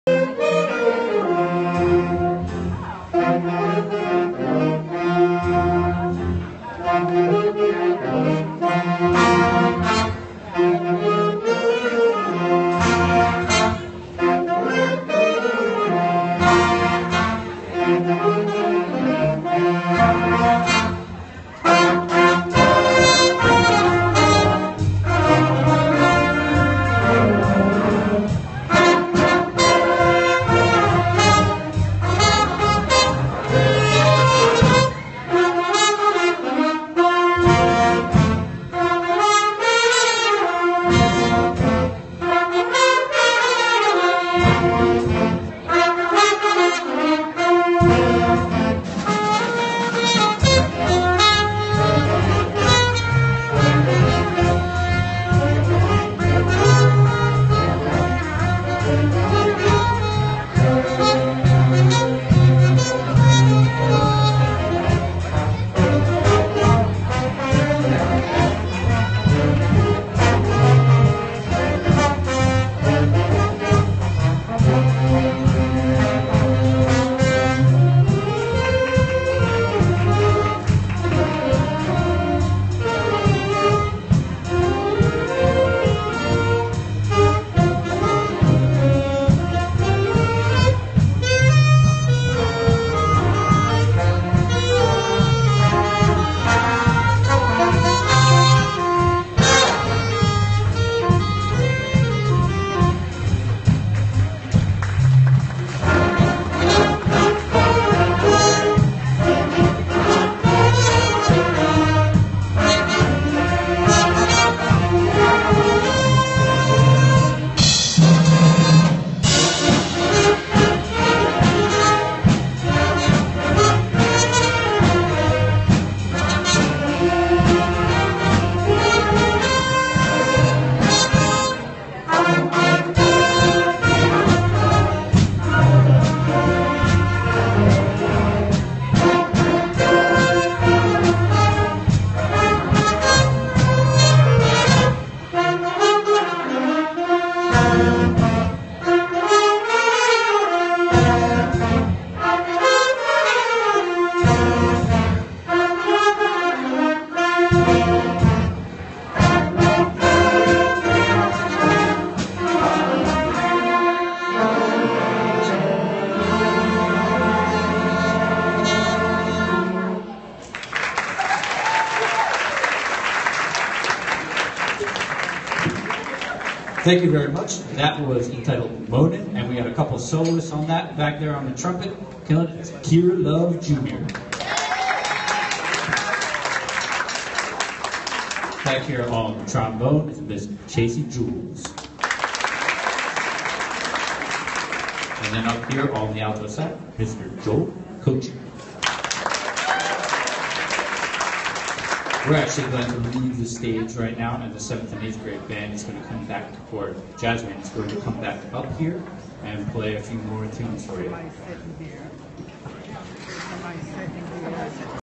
Pottstown Middle School Jazz II
The 5th and 6th Grade Jazz Band from Pottstown Middle School perform at the Jazz and Pancake Breakfast fundraiser at Pottstown High School on Saturday, April 23, 2016.